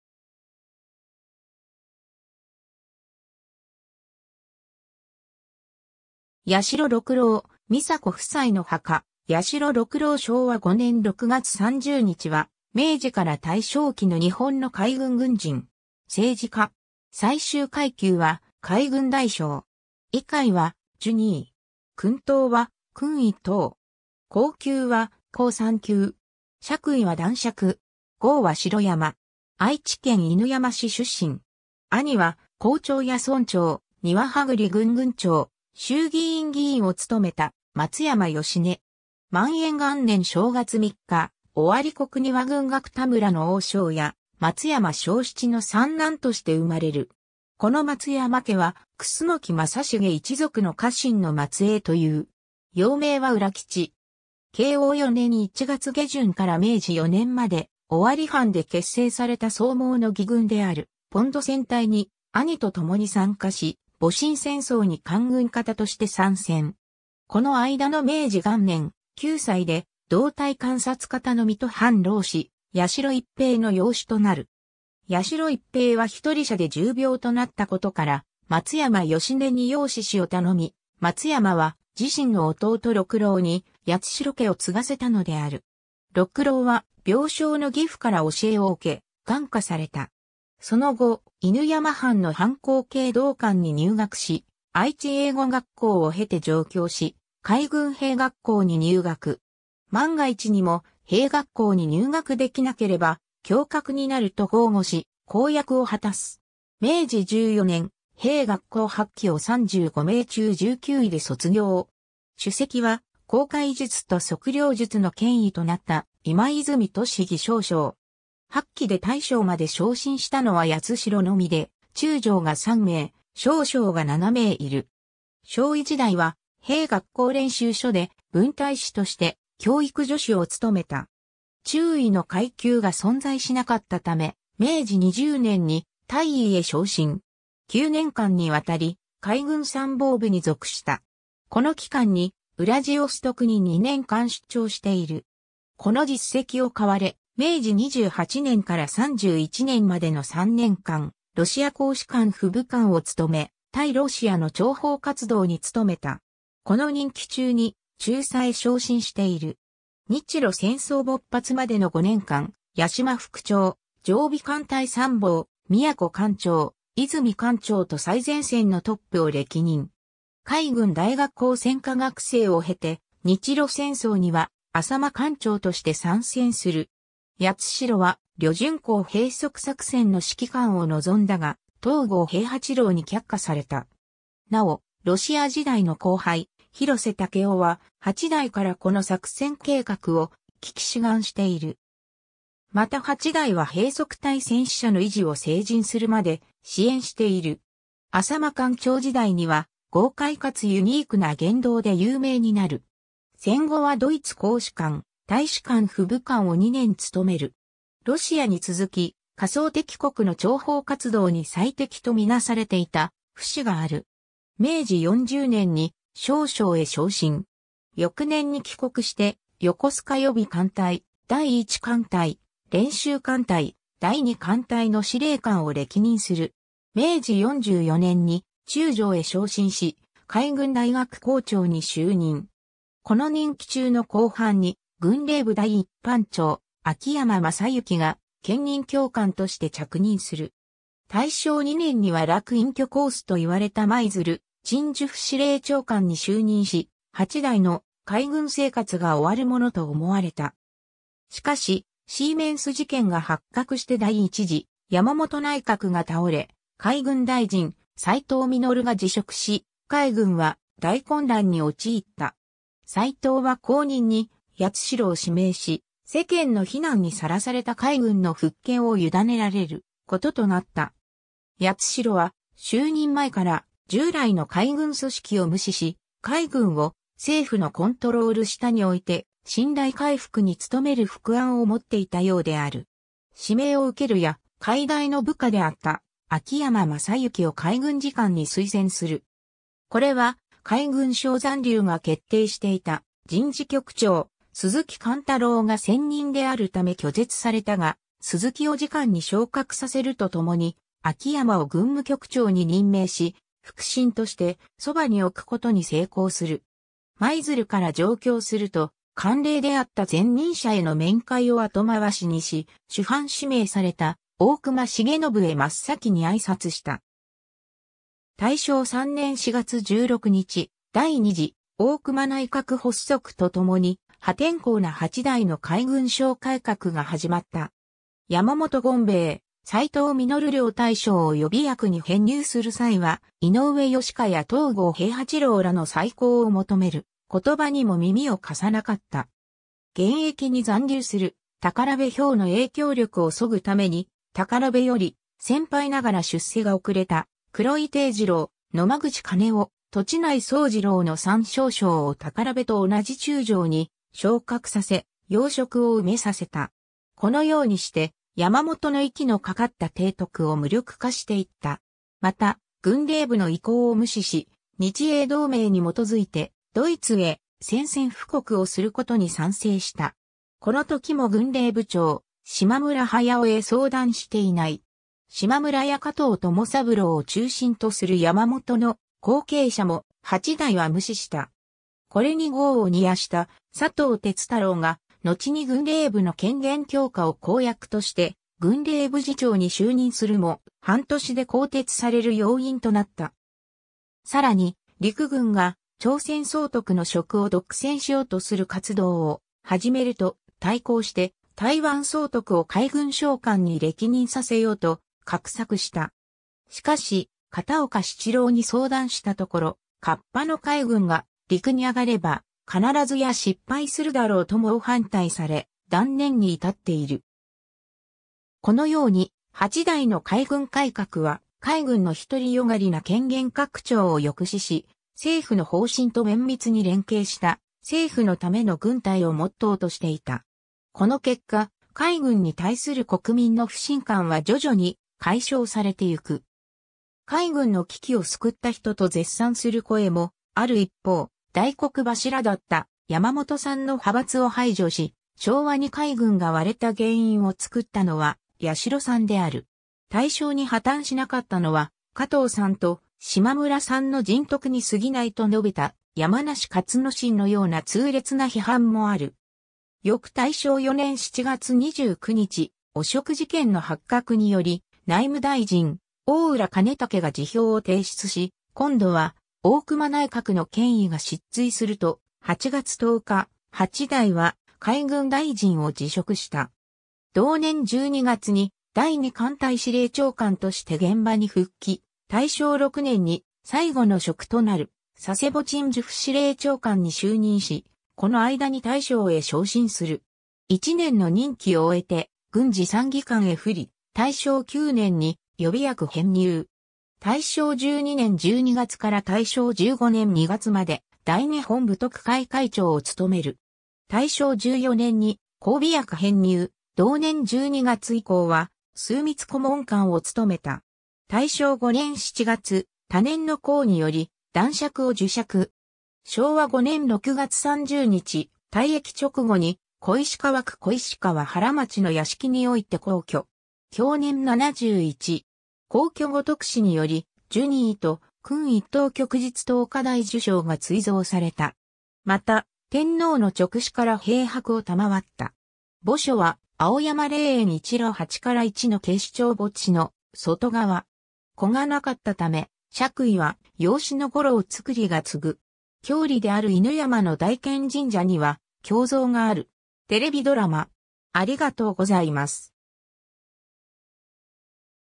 八代六郎
八代六郎・操子夫妻の墓 八代 六郎（やしろ ろくろう、安政7年1月3日（1860年1月25日）- 昭和5年（1930年）6月30日）は、明治から大正期の日本の海軍軍人。
政治家。
 最終階級は海軍大将。
位階は従二位。
勲等は勲一等。
功級は功三級。
爵位は男爵。
号は城山。
 愛知県犬山市出身。
兄は校長や村長、丹羽葉栗郡郡長、衆議院議員を務めた松山義根。
 万延元年正月三日(墓誌によれば四日)、尾張国丹羽郡楽田村（犬山市）の大庄屋、松山庄七の三男(墓誌によれば二男)として生まれる。
この松山家は、楠木正成一族の家臣の末裔という。
幼名は浦吉。
慶応4年1月下旬から明治4年まで、尾張藩で結成された草莽の義軍である磅磚隊に兄と共に参加し、戊辰戦争に官軍方として参戦。
この間の明治元年（1868年）、9歳で同隊監察方の水戸藩浪士(天狗党)・八代逸平の養子となる。
八代逸平は一人者で重病となったことから、松山義根に養嗣子を頼み、松山は自身の弟六郎に八代家を継がせたのである。
六郎は病床の義父から教えを受け、感化された。
その後、犬山藩の藩校敬道館に入学し、愛知英語学校(旧制愛知一中、現在の愛知県立旭丘高校)を経て上京し、海軍兵学校に入学。
「万が一にも兵学校に入学できなければ侠客になる」と豪語し、公約を果たす。
 明治14年（1881年）、兵学校8期を35名中19位で卒業。
首席は航海術と測量術の権威となった今泉利義少将。
8期で大将まで昇進したのは八代のみで、中将が3名・少将が7名いる。
少尉時代は兵学校練習所で分隊士として教育助手を務めた。
中尉の階級が存在しなかったため、明治20年（1887年）に大尉へ昇進。
9年間にわたり海軍参謀部に属した。
 この期間にウラジオストクに2年間出張している。
この実績を買われ、明治28年（1895年）から31年までの3年間、ロシア公使館附武官を務め、対ロシアの諜報活動に努めた。
この任期中に中佐へ昇進している。
 日露戦争勃発までの5年間、「八島」副長、常備艦隊参謀、「宮古」艦長、「和泉」艦長と最前線のトップを歴任。
海軍大学校選科学生を経て、日露戦争には「浅間」艦長として参戦する。
八代は旅順港閉塞作戦の指揮官を望んだが、東郷平八郎に却下された。
なお、ロシア時代の後輩・広瀬武夫は、八代からこの作戦計画を聞き志願している。
また八代は閉塞隊戦死者の遺児を成人するまで支援している。
 浅間艦長時代には、豪快かつユニークな言動で有名になる。
 戦後はドイツ公使館・大使館附武官を2年務める。
ロシアに続き仮想敵国の諜報活動に最適とみなされていた節がある。
明治40年（1907年）に少将へ昇進。
翌年に帰国して横須賀予備艦隊、第1艦隊、練習艦隊、第2艦隊の司令官を歴任する。
 明治44年（1911年）に中将へ昇進し、海軍大学校長に就任。
この任期中の後半に、軍令部第1班長・秋山真之が兼任教官として着任する。
大正2年（1913年）には楽隠居コースと言われた舞鶴鎮守府司令長官に就任し、八代の海軍生活が終わるものと思われた。
 しかしシーメンス事件が発覚して第1次山本内閣が倒れ、海軍大臣・斎藤実が辞職し、海軍は大混乱に陥った。
斎藤は後任に八代を指名し、世間の非難にさらされた海軍の復権をゆだねられることとなった。
 八代は就任前から、従来の海軍組織を無視し、海軍を政府のコントロール下に置いて信頼回復に努める腹案を持っていたようである。
指名を受けるや、海大の部下であった秋山真之を海軍次官に推薦する。
これは海軍省残留が決定していた人事局長・鈴木貫太郎が先任であるため拒絶されたが、鈴木を次官に昇格させるとともに、秋山を軍務局長に任命し、腹心としてそばに置くことに成功する。
舞鶴から上京すると、慣例であった前任者への面会を後回しにし、首班指名された大隈重信へ真っ先に挨拶した。
 大正3年（1914年）4月16日、第2次大隈内閣発足とともに、破天荒な八代の海軍省改革が始まった。
山本権兵衛・斎藤実両大将を予備役に編入する際は、井上良馨や東郷平八郎らの再考を求める言葉にも耳を貸さなかった。
現役に残留する財部彪の影響力をそぐために、財部より先輩ながら出世が遅れた黒井悌次郎・野間口兼雄・栃内曽次郎の3少将を財部と同じ中将に昇格させ、要職を埋めさせた。
このよ...